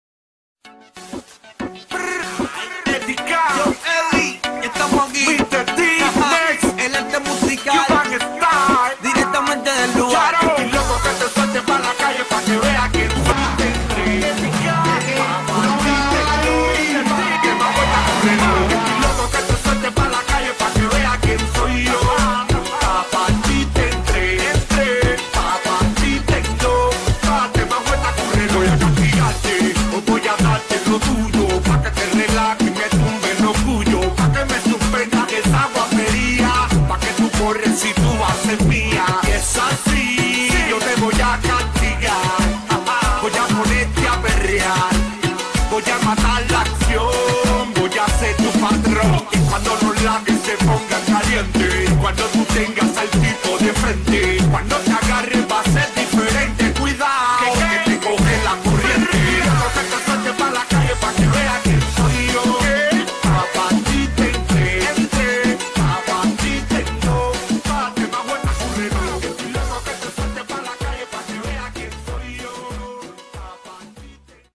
EDDY K y su grupo en concierto - @ MULATA LATIN SOCIAL CLUB EN BARI - Mar. 11 dic. 2012
tu MULATTIA  ... puro reggaeton ... La Medicina Que Cura el Alma
UNETE AL MOVIMIENTO